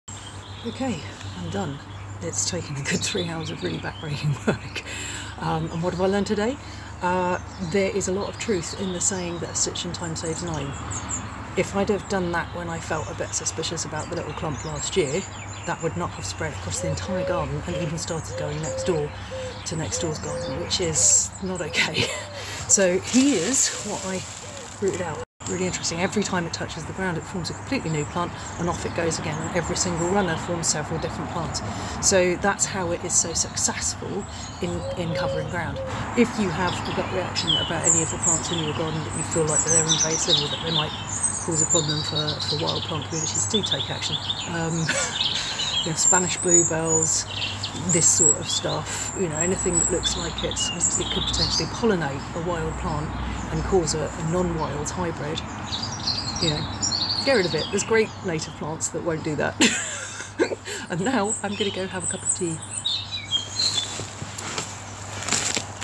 Tackling hybrid 'Aluminium' Archangel - an invasive weed in the UK! 3/3
Hello everyone! Here's part three of a three-part mini-series on tackling a hybrid form of Yellow Archangel, the brutish felon Aluminium Archangel, (Lamium galeobdolon subsp argentatum) which seeded in the garden from the native variety pollinated by a wild bee or other little person. In British gardening for wildlife, we just can't let that ride - other areas may have similar invasives!

Some folks have raised an issue with sound - CC is there if you need it, or here's the transcript:

OK, I'm done! It's taken a good three  hours of really back-breaking work, and what have I learned today? There is a lot  of truth in the saying that 'A stitch in time saves nine.' If I'd have done that when I felt a  bit suspicious about the little clump last year, that would not have spread across the entire  garden and even started going next door, to next door's garden, which is not  okay. So here is what I rooted out. Really interesting - every time it touches the  ground it forms a completely new plant, and off it goes again. And every single runner forms  several different plants, so that's how it is so successful in covering ground. If you have a gut  reaction about any of the plants in your garden, that you feel like they're invasive and that they  might cause a problem for wild plant communities, do take action! Spanish bluebells, this sort  of stuff... anything that looks like it could potentially pollinate a wild plant and cause  a a non-wild hybrid, you know, get rid of it. There's great native plants that won't do that.  And now I'm going to go and have a cup of tea!

Yellow Archangel (L. galeobdolon) is a great woodland plant in the UK with lovely yellow flowers, but the silvery-leaved garden variety is a menace, and listed on Schedule 9 of the UK Wildlife and Countryside Act. That's the Seriously Naughty Step for plants. The way it turned up uninvited made me worry it could also escape to pollinate Yellow Archangel out in the wild - which would be rubbish, as it smothers wild plants. Basics for wildlife gardening simplified - invasive plants aren't worth the risk in your garden, so dig 'em out!

Please click on the links for parts 1 and 2.

Happy wildlife gardening, folks! :)